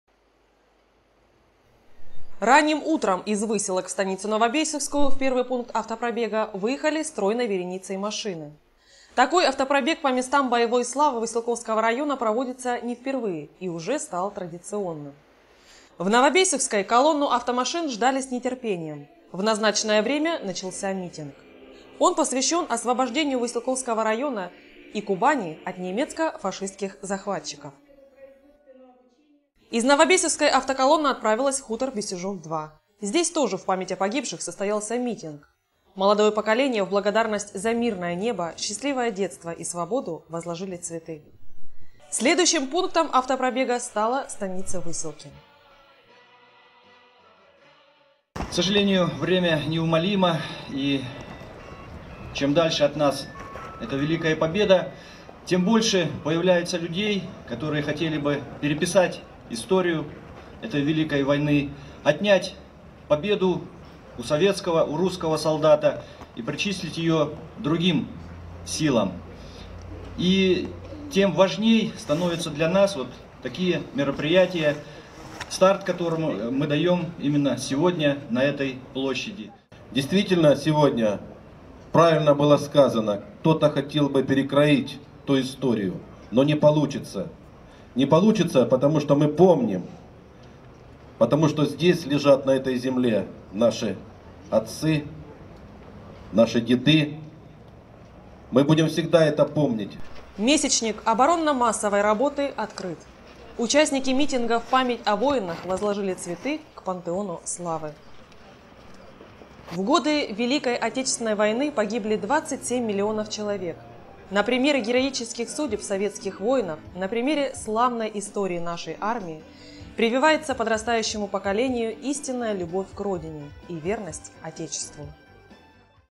Автопробег
На Кубани 24 января стартовал месячник оборонно-массовой и  военно-патриотической работы под общим девизом Кубань - край ратной и воинской славы. В Выселковском районе в рамках месячника будет проводиться целый ряд мероприятий: встречи с ветеранами, уроки мужества, а также спортивные мероприятия. В понедельник состоялся автопробег по местам боевой славы и торжественные митинги.